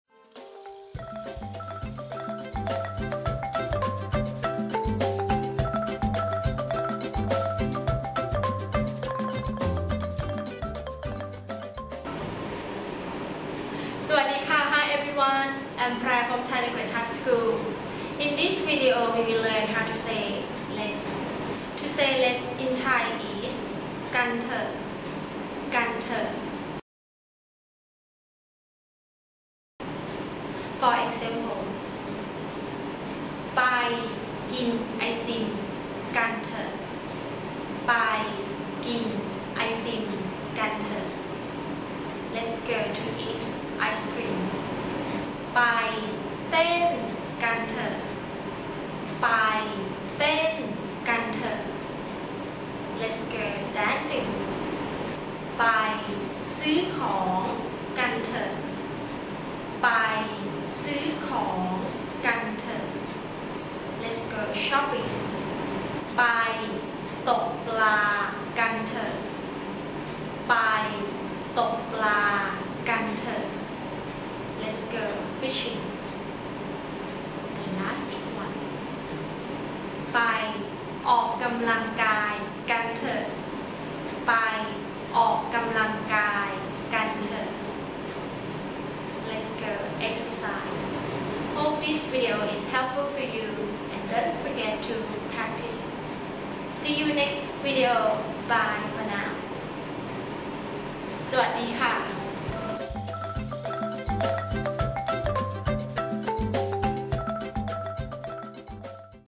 Learn Thai Phrase | Let's | กันเถอะ /gan tùh/
If you like this lesson, please click on the "Like" botton. In this video, we will learn how to say "Let's" ,กันเถอะ /gan tùh/. This is a widely used phrase.
We do lessons at our school, at a site of your choice in Bangkok or learn Thai Online using Skype.